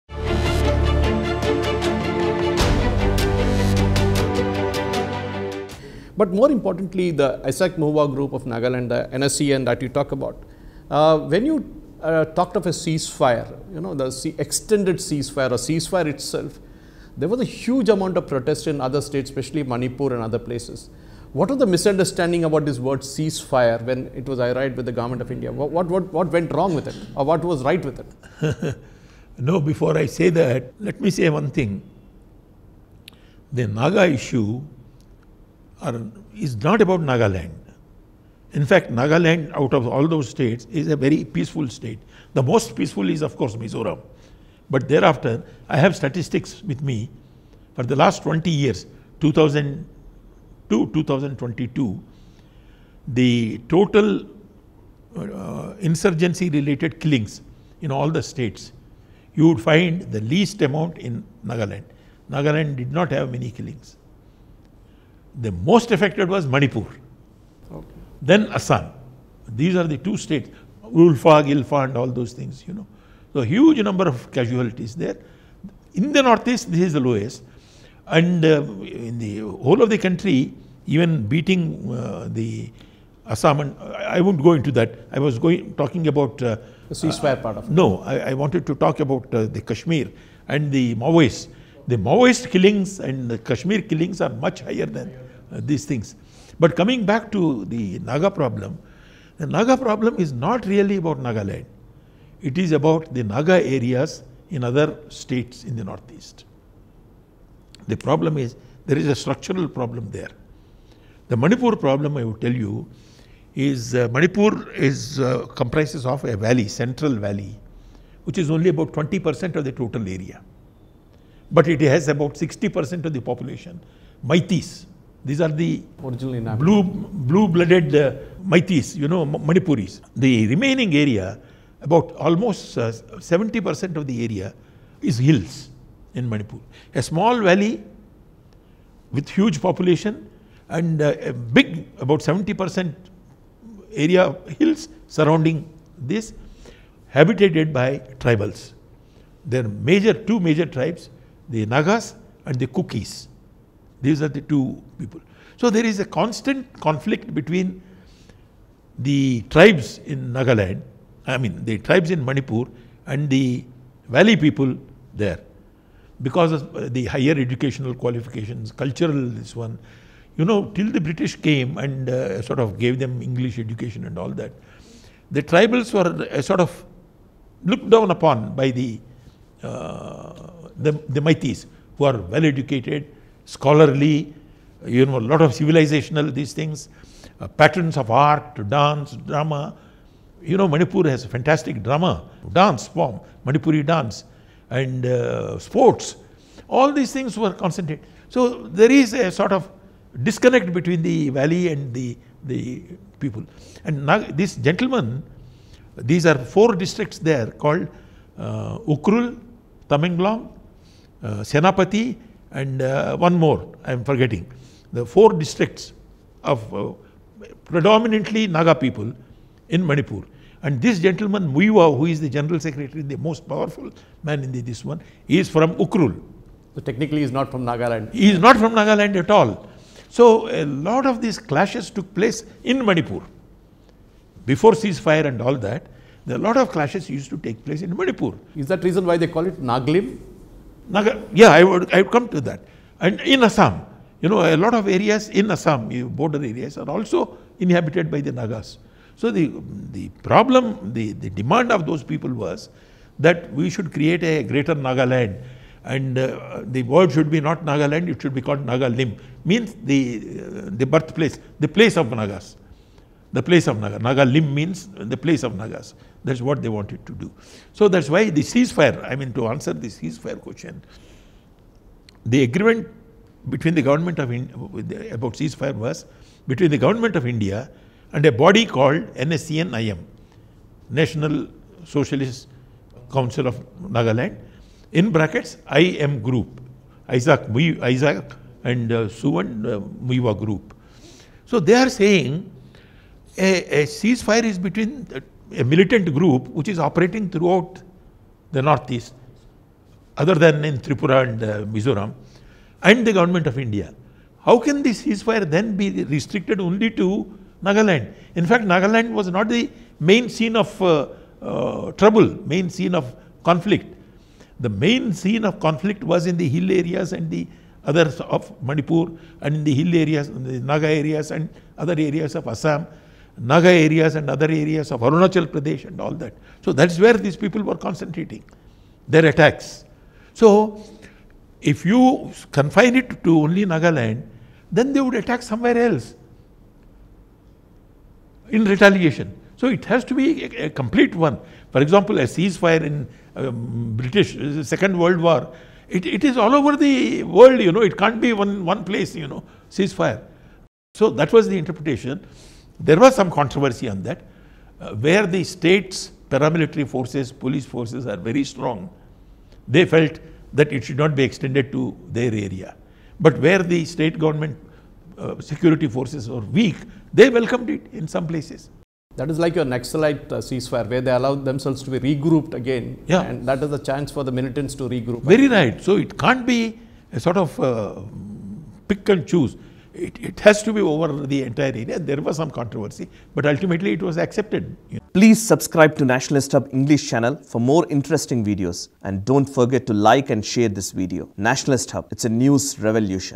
Ceasefire in Nagaland and Separatists in North East States | K.Padmanabhaiah |NationalistHub English
Our Company account details :
Name : DHARMAGNYA BROADCASTING PVT LTD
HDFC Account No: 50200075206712
IFSC Code: HDFC0000512